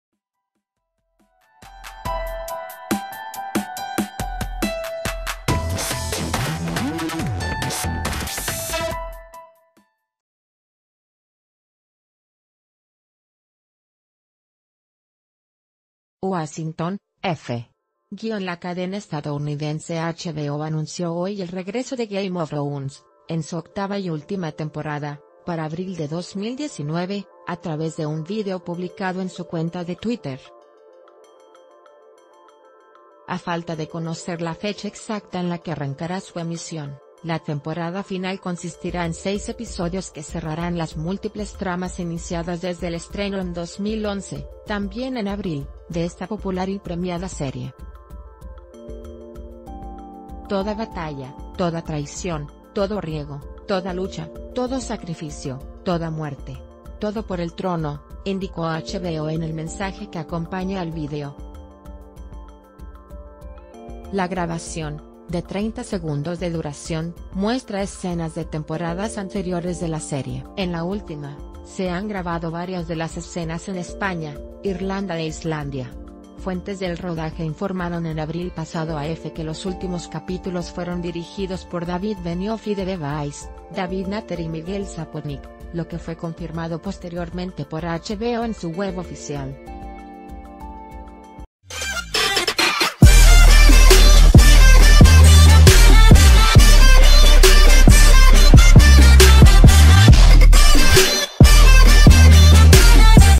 La última temporada de “Game of Thrones” se estrenará en abril de 2019 - El Diario de Yucatán
Washington (EFE).- La cadena estadounidense HBO anunció hoy el regreso de “Game of Thrones“, en su octava y última temporada, para abril de 2019, a través de un vídeo publicado en su cuenta de Twitter. A falta de conocer la fecha exacta en la que arrancará su emisión, la temporada final consistirá e...